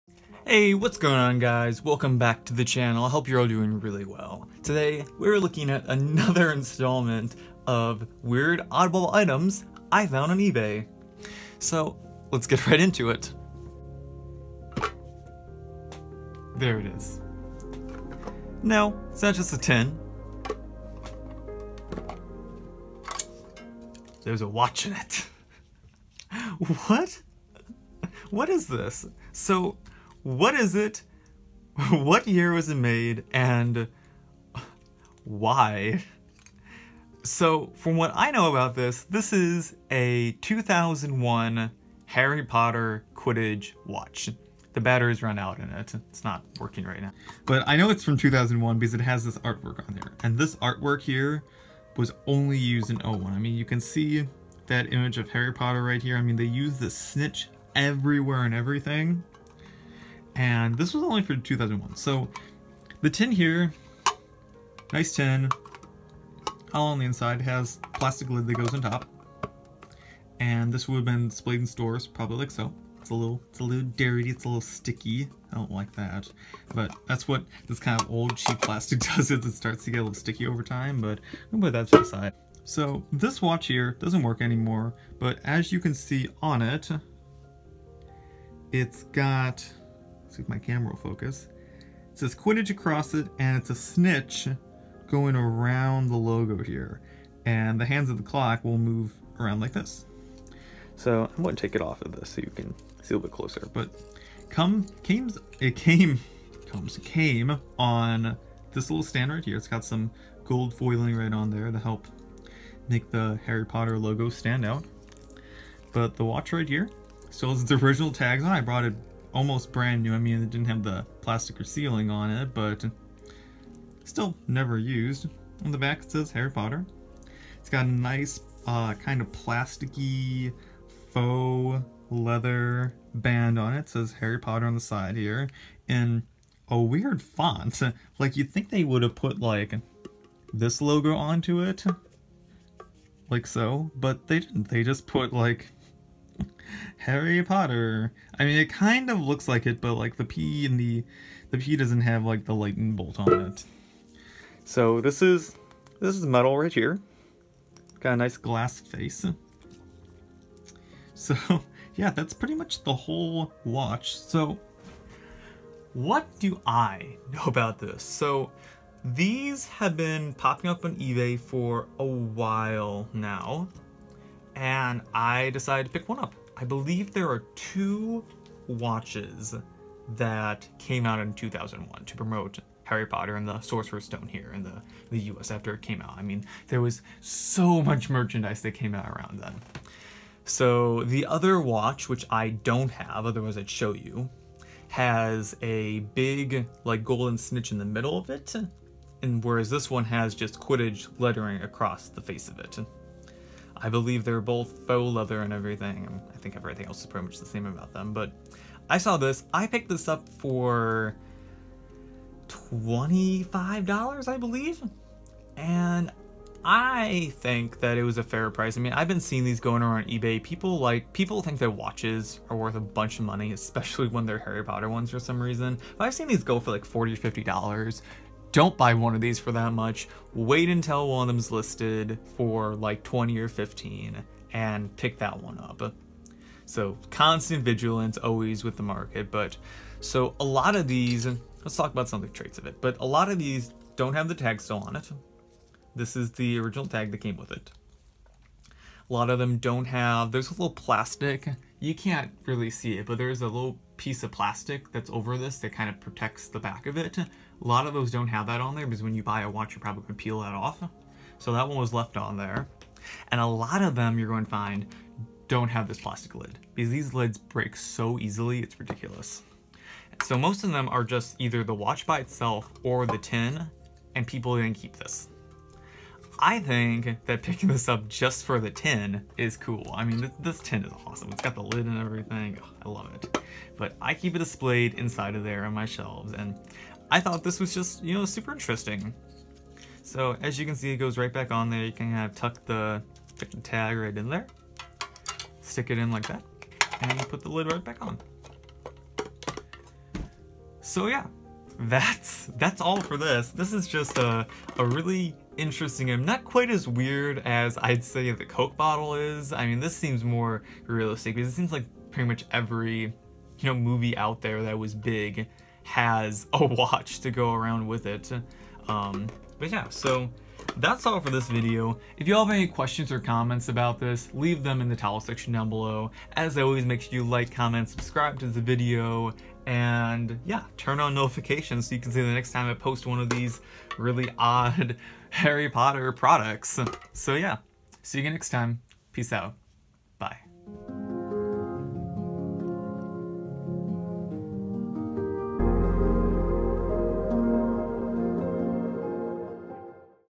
Harry Potter 2001 Watch Review!
This cool watch was made back in 2001 to promote Harry Potter and the Philosopher Stone/Sorcerer’s Stone. I hope you enjoyed this video!